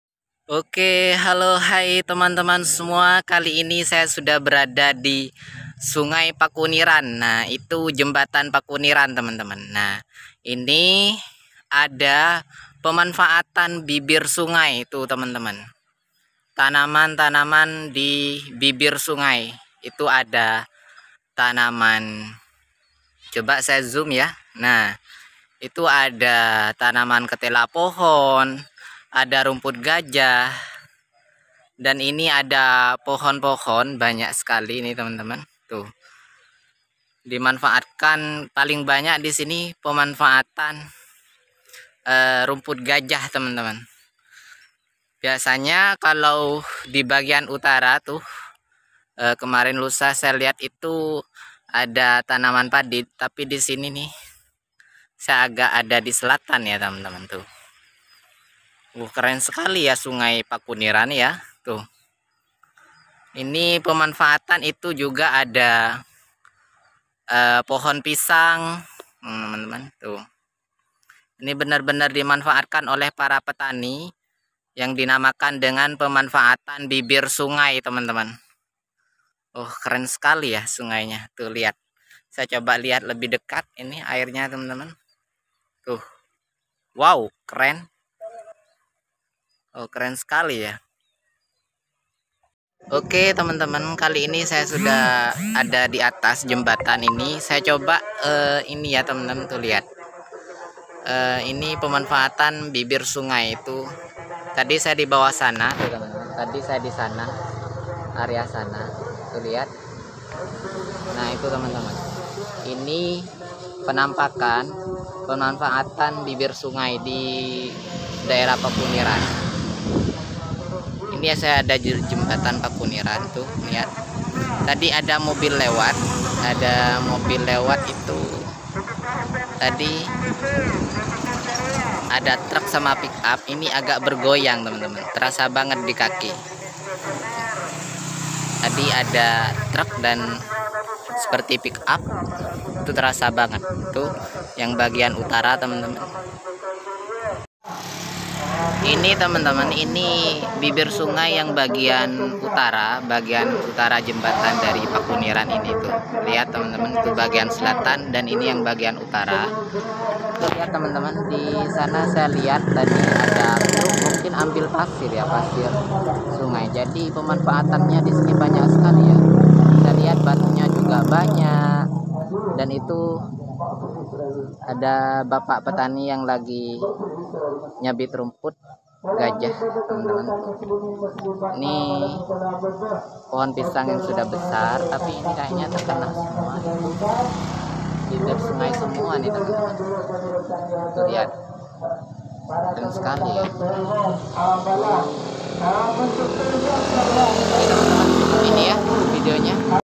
Pemanfaatan Bibir Sungai
halo kawan-kawan semua

Jumpa lagi bersama kami asosiasi petani

Video ini tentang Pemanfaatan Bibir Sungai

Simak videonya sampai habis ya karena sangat bermanfaat